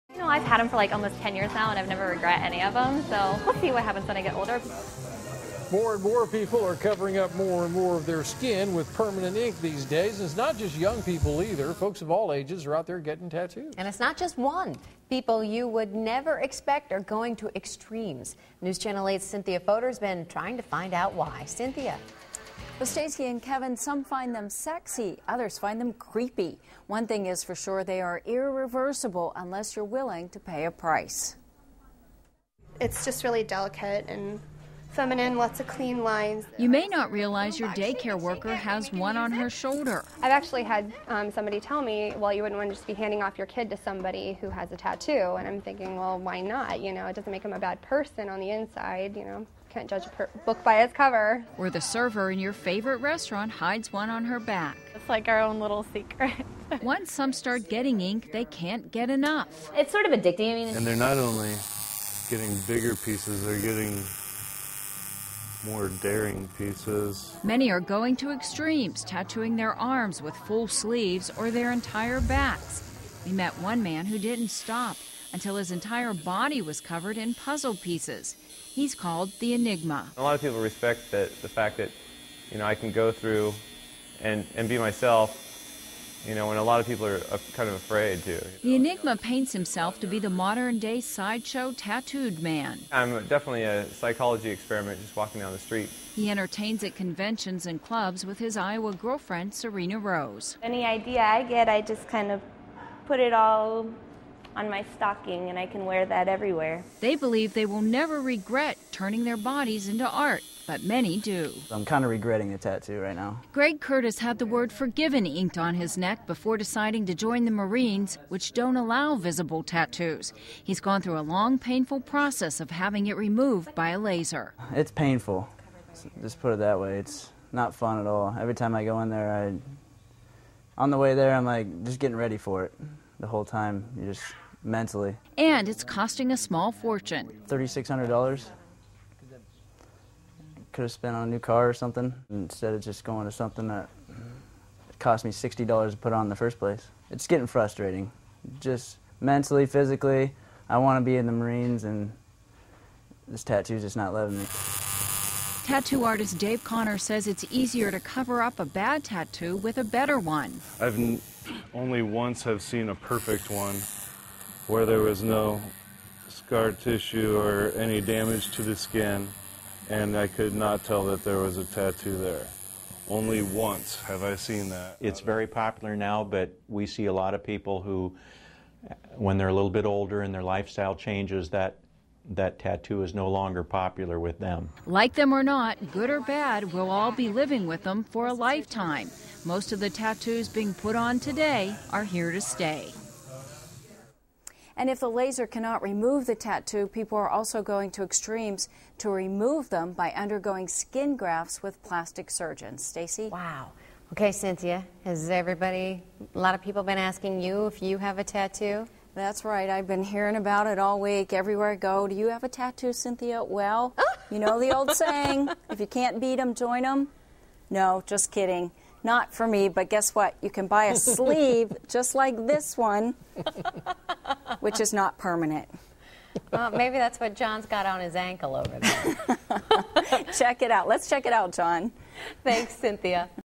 Special Report: Tattoos, Removals Hot In Iowa
Some people in Iowa are going to extremes to get tattoos and to have them removed.